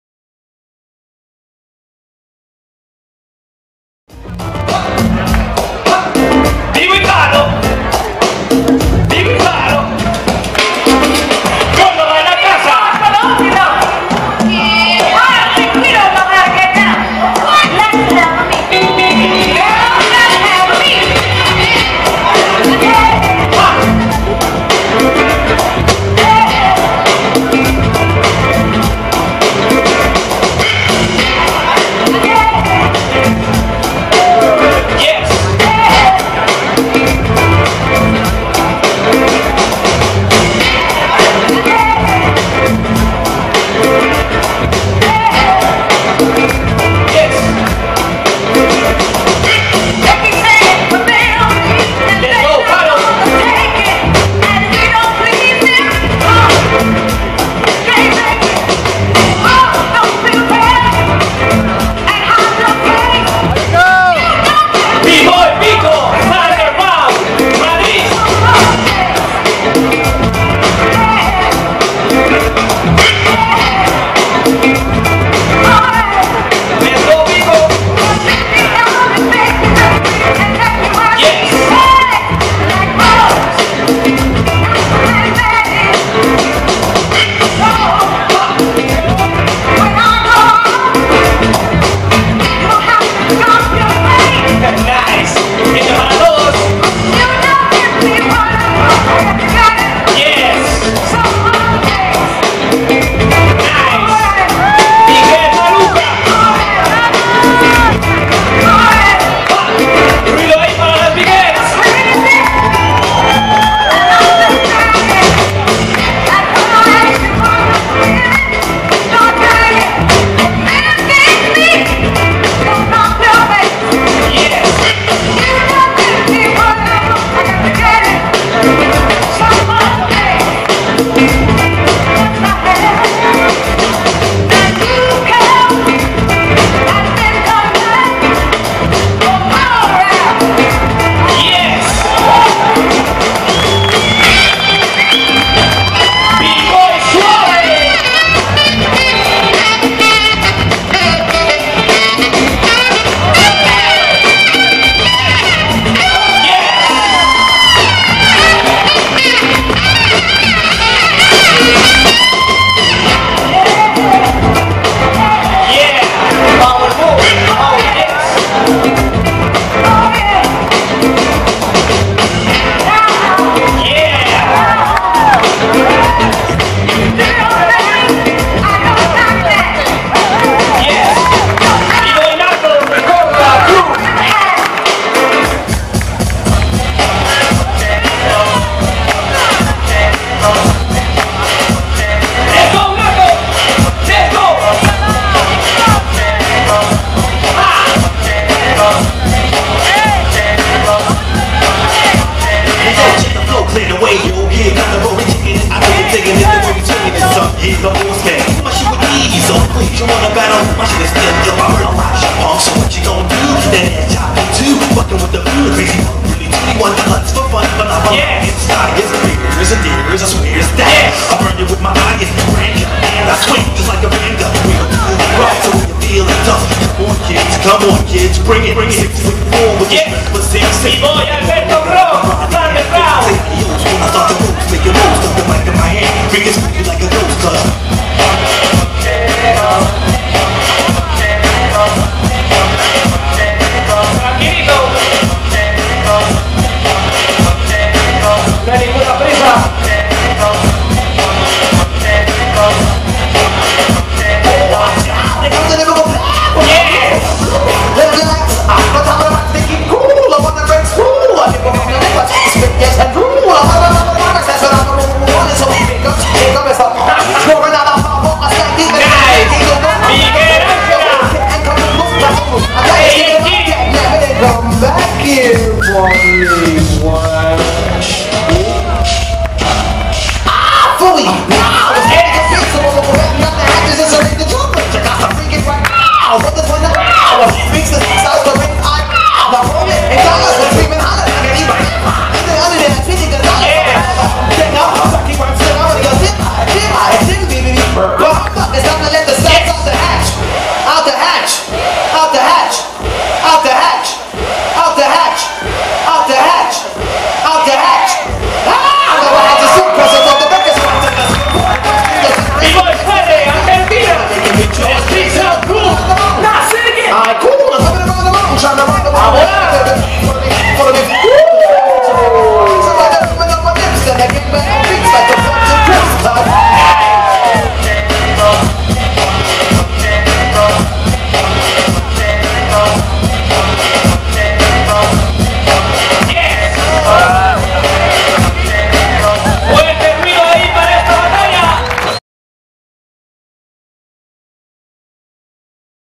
A Ras del Suelo 2019 - 06 Filtros
A RAS DEL SUELO 2019 2vs2.
06 Filtros - Halo & Nato vs Albertop Rock & Viko vs A.N.G. & Maluka vs Fede & Suave
DJ: Jimmy Twice (Madrid King Flava/99 Flava) Madrid
JUECES
Hulee (Fusion Rockers/Madrid King Flava) Madrid
Jorge (Special K/King Cobras/XVIIgeneration) Valencia
Nuclearbat (Pressing Danz) Madrid
GANADORES
Primer puesto: G Round & Minijoe
Segundo Puesto: Jose´Ra & Amado
PREMIOS
- 1ª Premio: 400€ (tarjetas valoradas en 200€ por Bboy)
- 2ª Premio: 200€ (tarjetas valoradas en 100€ por Bboy)
- Mejor Bboy: 50€ en tarjeta + gafas de sol de STEENBOLK de la Classic Collection